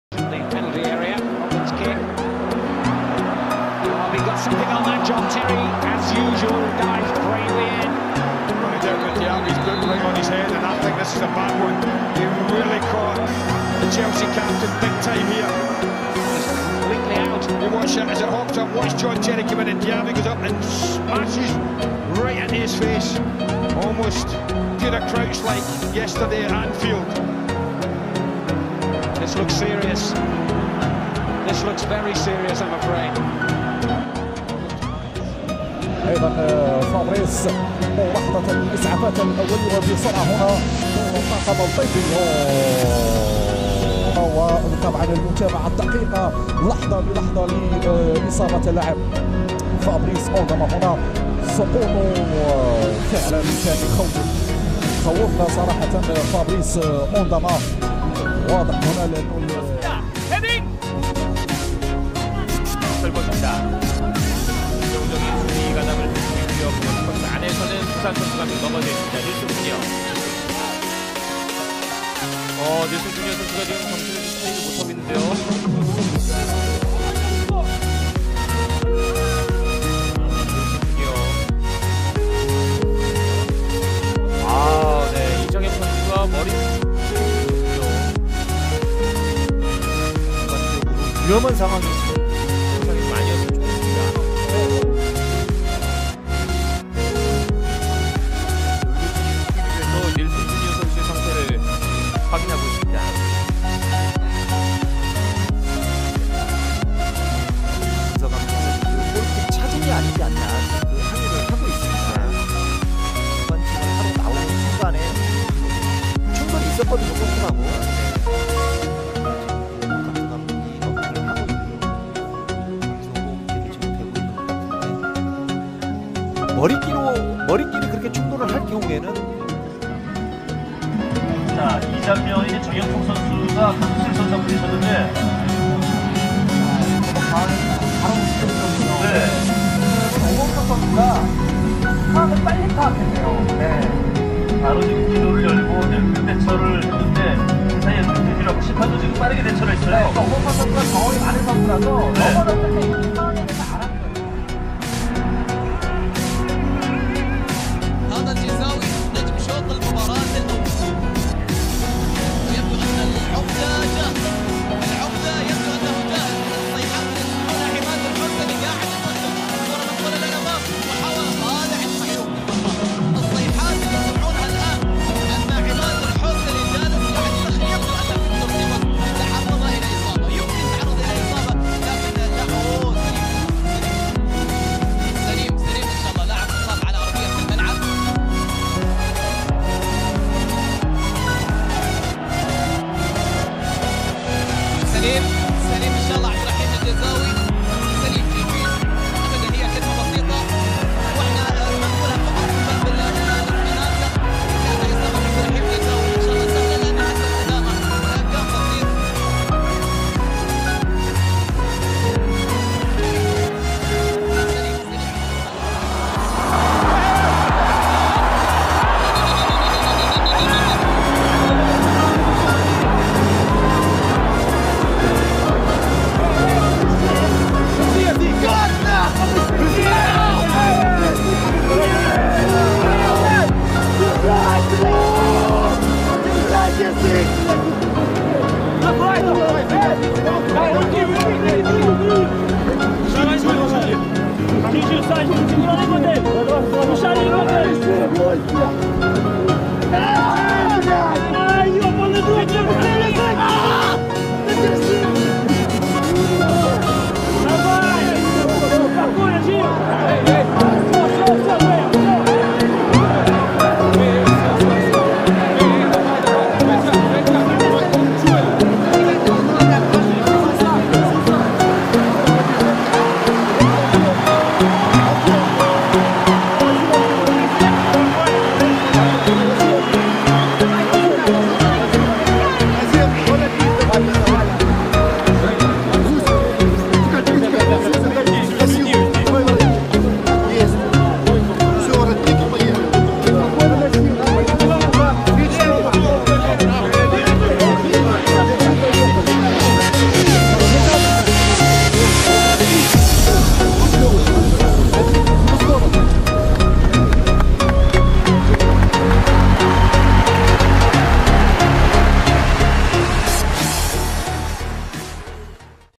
Players who rescue opponents life /hd/heroes
This video is about players who almost die  on ground
Thank you for your support and click on the like button have a nice day 👍